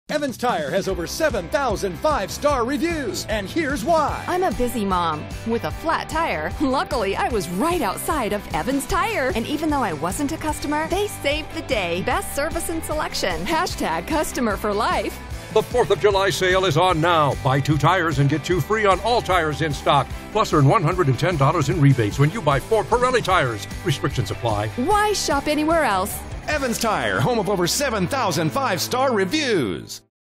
EvansTire & Service Centers B2G2 Pirelli 4th of July Savings
Evans Tire - Home of over 7,000 5-Star Reviews has started our 4th of July sale! Come in today and Save during our All In Stock tire Sale!
Take advantage of our Pirelli Tire Special, Buy 4 Tires and get up to $110 in rewards!